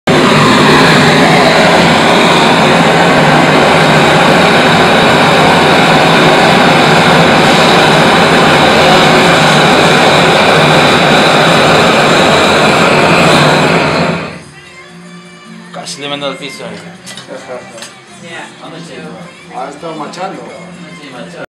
Molten silver in Cuzco
Making silver ingots to use for jewelry at the silversmith's in Cuzco.